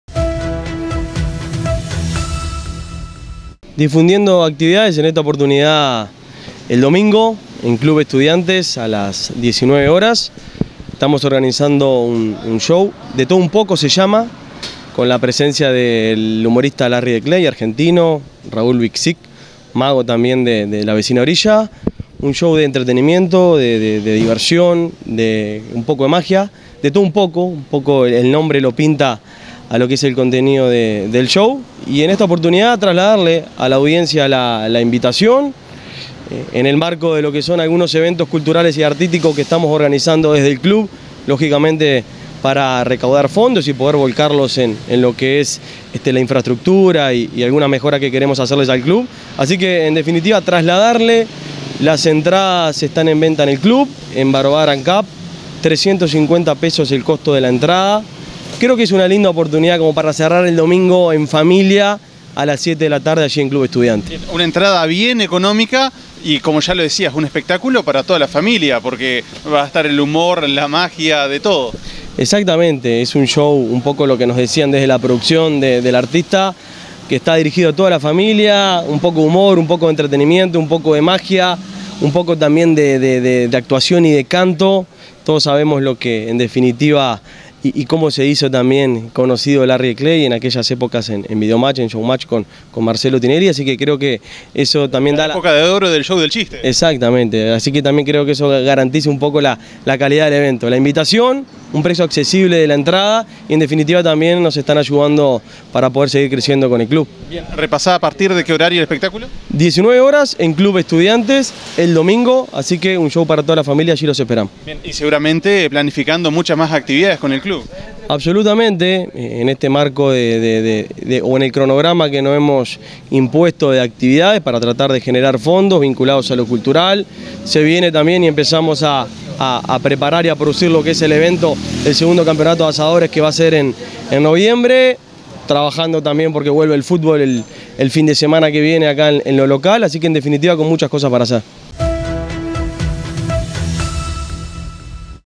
"De todo un poco" se denomina el evento con Larry de Clay y Raúl Biczik que se realizará en Tbó
El mismo es mpulsado por el Club Estudiantes. Será este domingo 6 de agosto, a las 19:00 horas. Hablamos con Augusto Souza